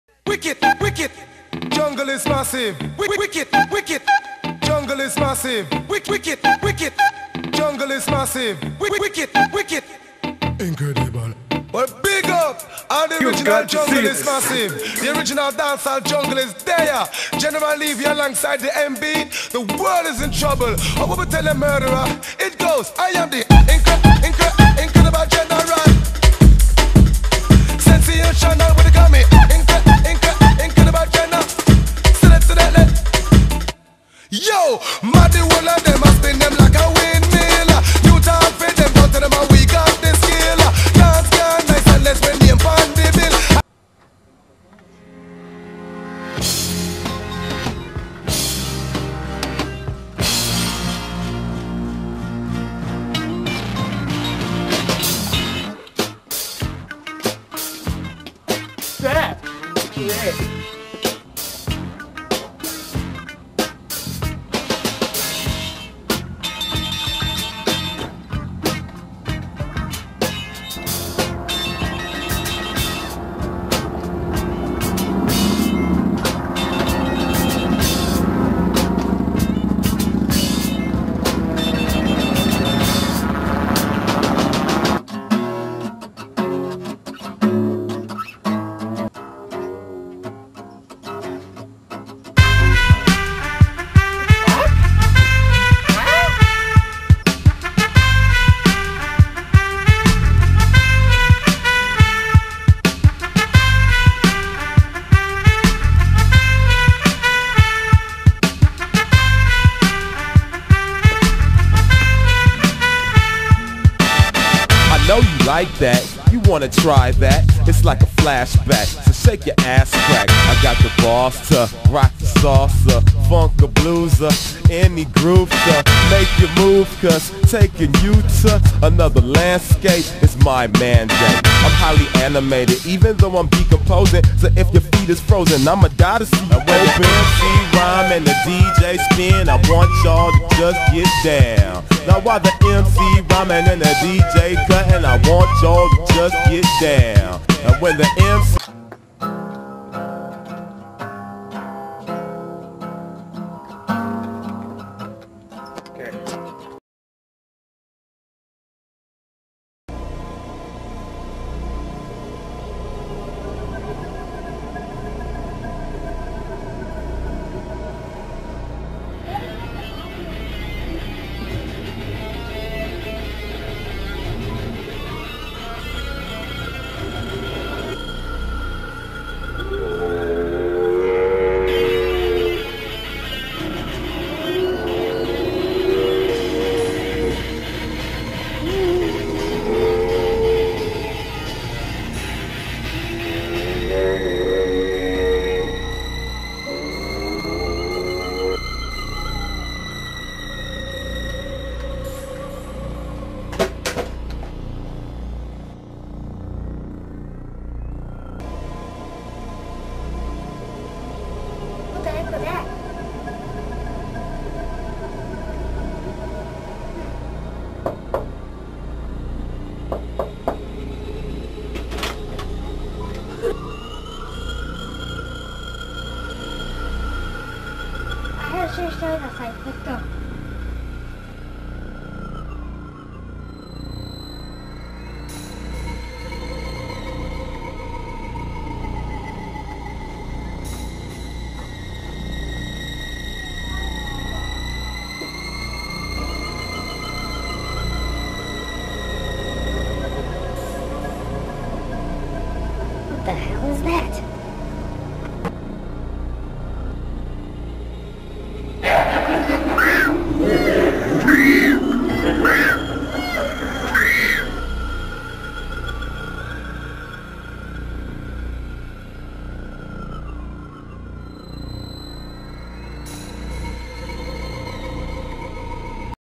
Tony Pitts has some fresh moves
A man who dances to the tunes of life around him. Everyone seems to hear it though.